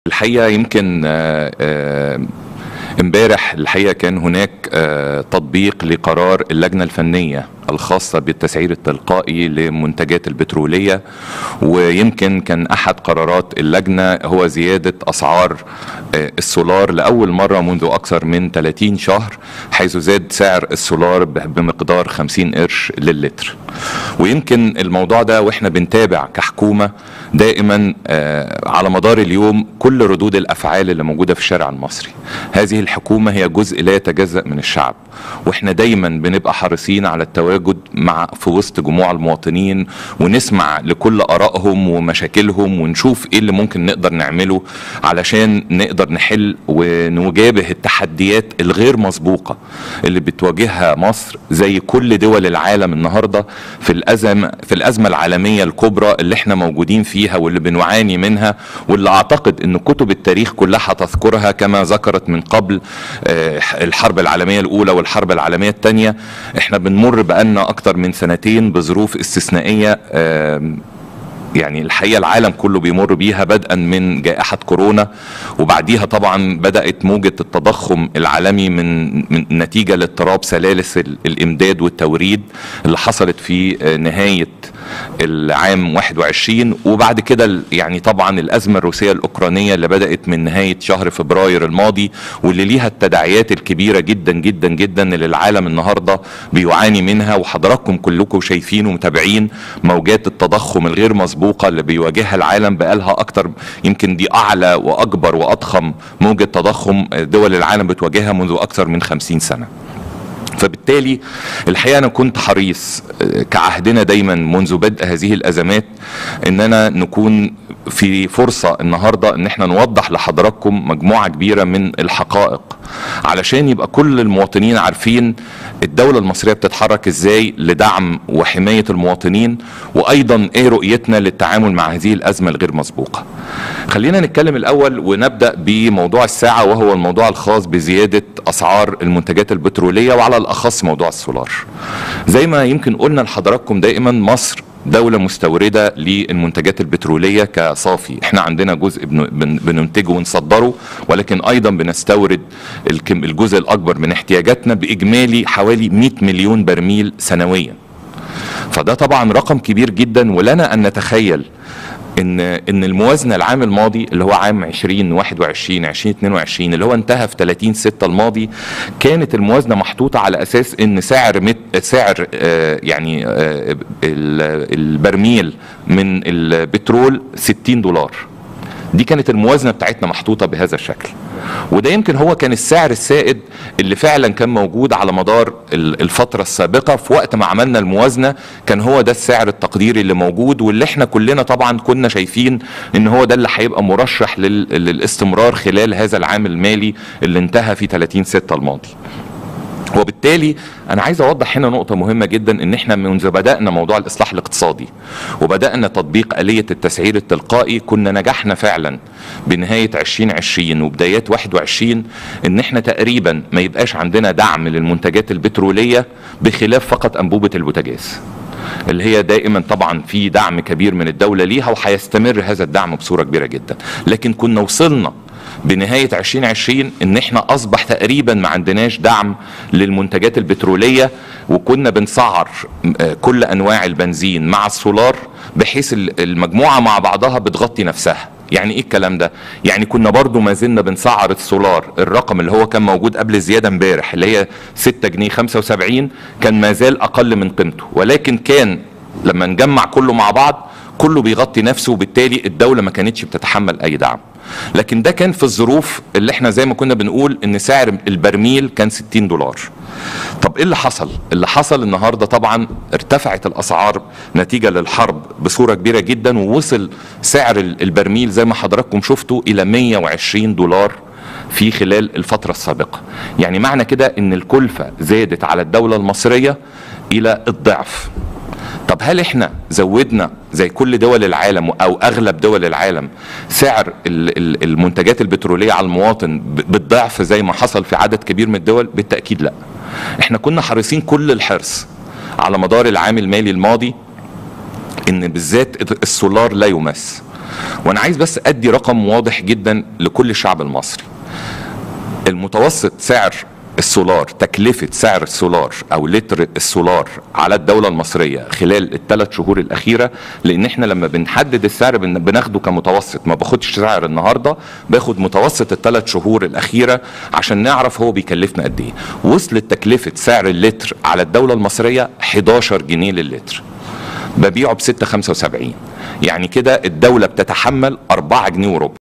مدبولي: تحريك أسعار السولار يهدف لمواجهة ارتفاع تكلفته عالميا.. والدولة لا تزال تتحمل جزءا من الدعم
قال رئيس الوزراء مصطفى مدبولي أن تحريك أسعار السولار يهدف لمواجهة ارتفاع تكلفته عالميا.. والدولة لا تزال تتحمل جزءا من الدعم